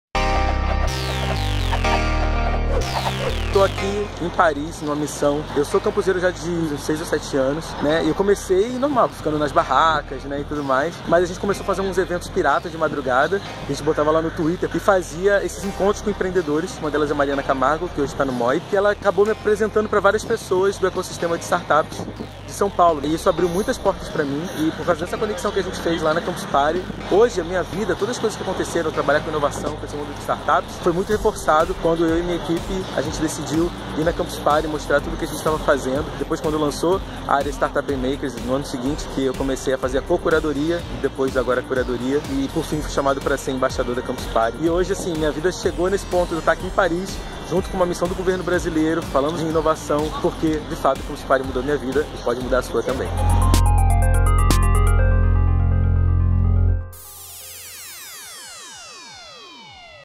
Como a Campus Party Mudou a Minha Vida
Vinícius Machado, embaixador da Campus Party, fala sobre suas experiências com o evento.